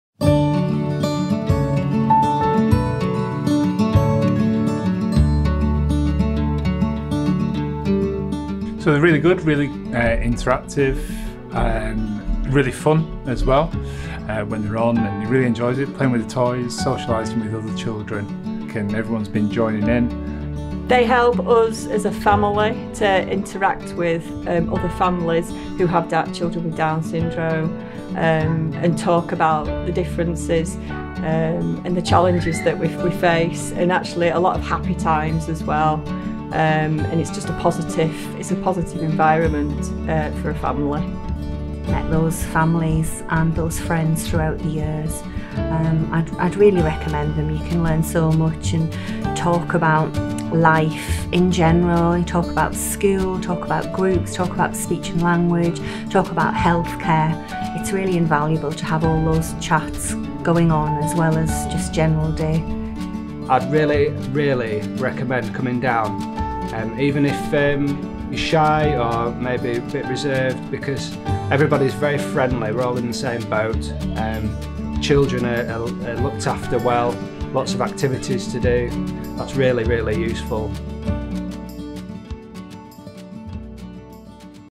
Saturday Club
Second Collaboration 2 with Bradford Down Syndrome Training & Support Service Ltd

This is a collaboration with Down Syndrome Training & Support Service Ltd to produce short promotional videos to be uploaded to social media. I filmed all the shots with the Sony A7 camera and edited with a Premiere Pro editing programme. Before the shoot, I made various promotional concepts through meetings with the public relations of the Down Syndrome Centre. I created storyboards and shot list myself, and based on that, I filmed.

Film and Edit by Jun Hong